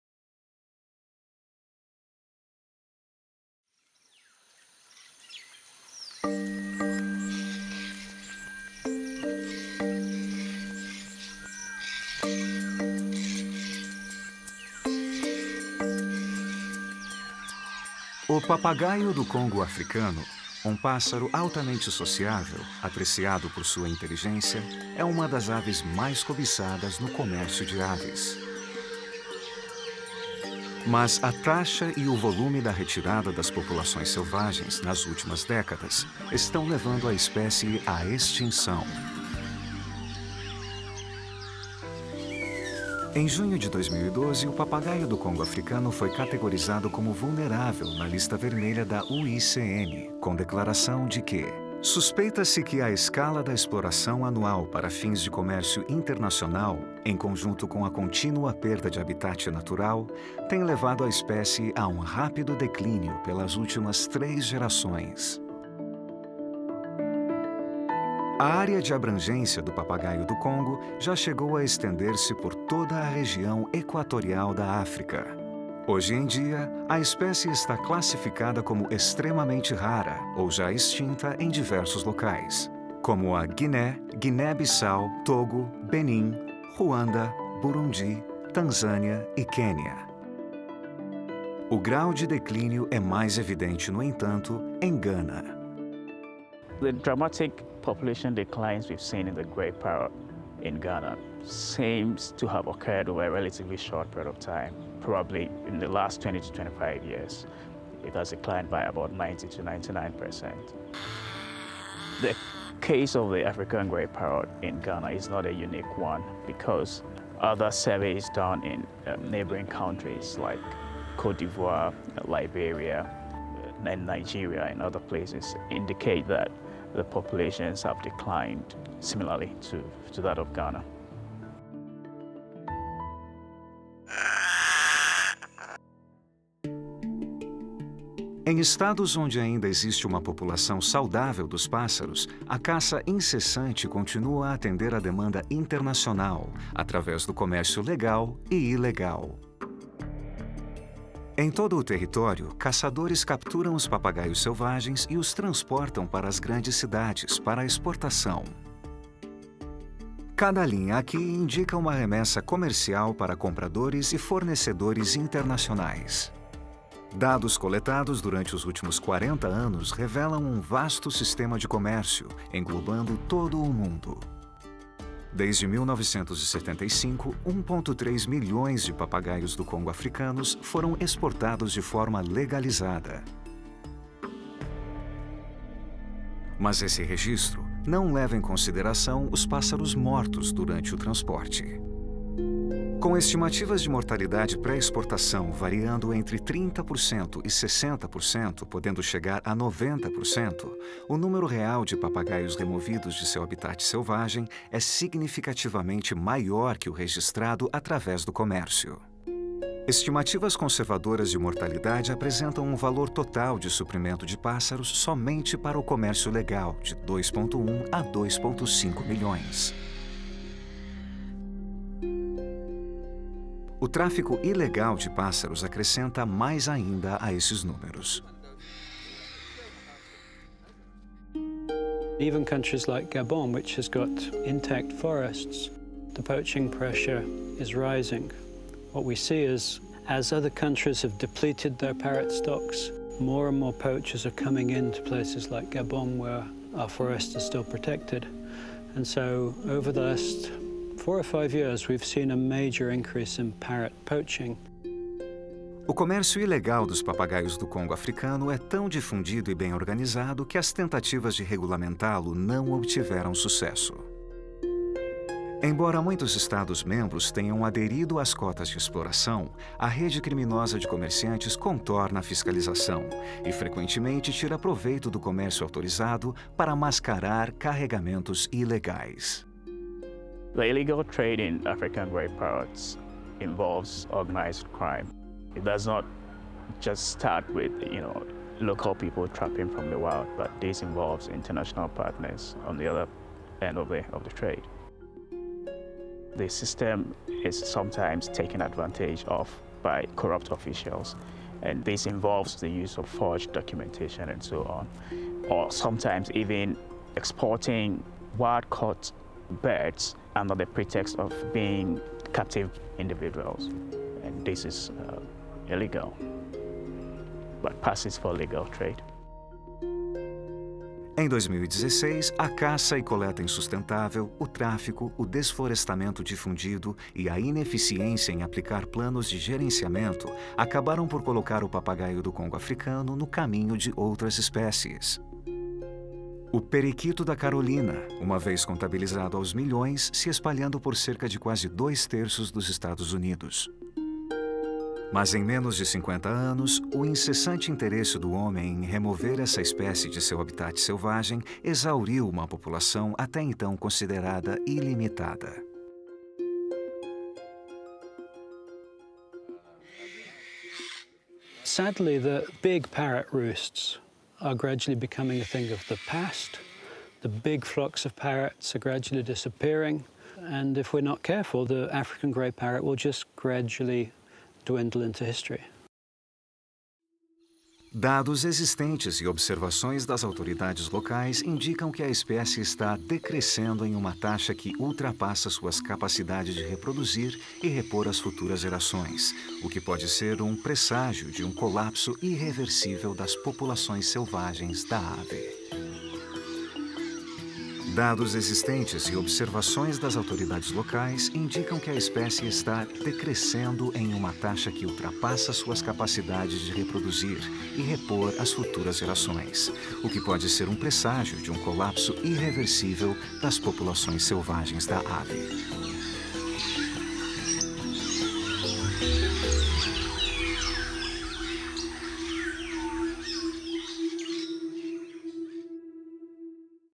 African Grey Parrot: Species in Decline (Portuguese)
The African Grey Parrot – a highly intelligent bird that is popular as a pet – has been eliminated from much of its west African range and the largest populations are now only found in central Africa. In the fall of 2016, delegates from around the globe will meet for the world’s leading forum to debate and discuss issues related to international wildlife trade - the 17th meeting of the Conference of the Parties (CoP17) to the Convention on International Trade in Endangered Species of Wild Fauna and Flora, or CITES. Parties will consider a proposal to transfer the African Grey Parrot (Psittacus erithacus) from Appendix II to Appendix I, effectively banning international commercial trade in the species.